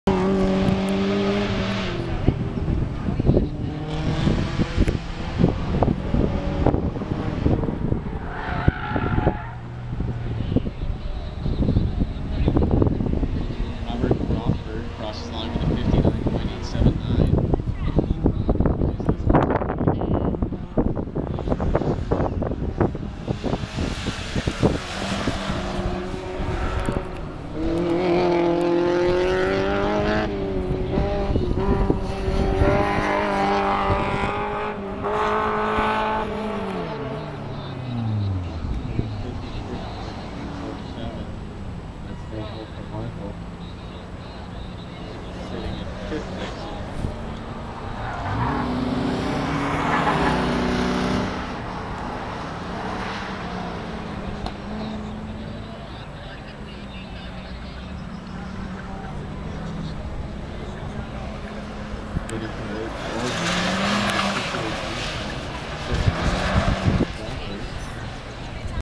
7/12/09 WDCR SCCA Autocross Championship Event #3
I have no idea what run this was. Overall, I finished 6th in class out of a tolal of 15.